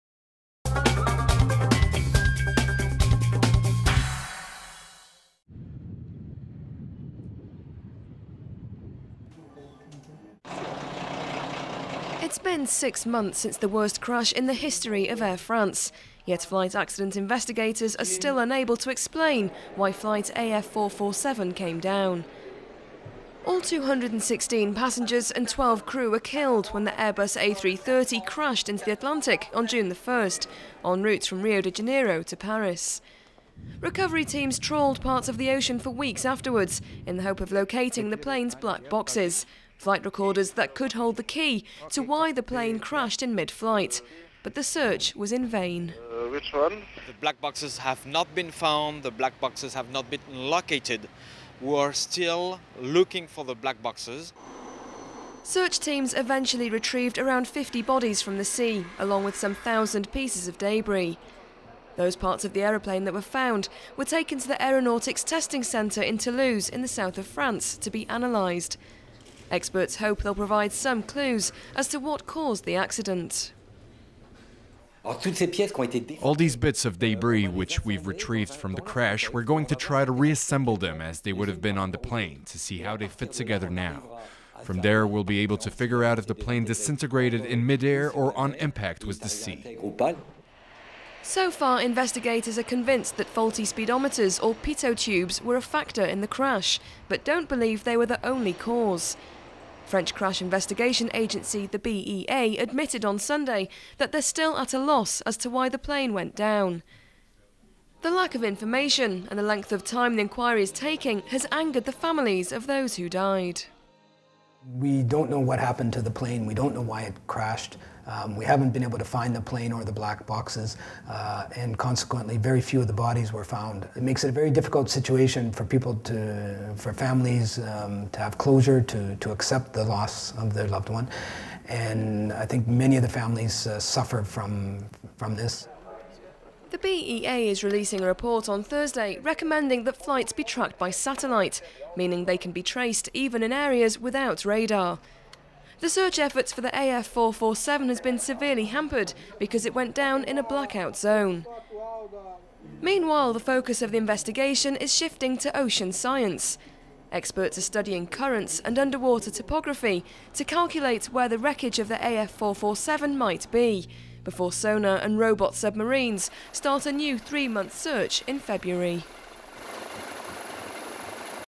AF447 flight: 6 months since the worst crash in the ...
PARIS-RIO FLIGHT: The BEA is releasing a report, recommending that flights be tracked by satellite, meaning that they can be traced even in areas without radar.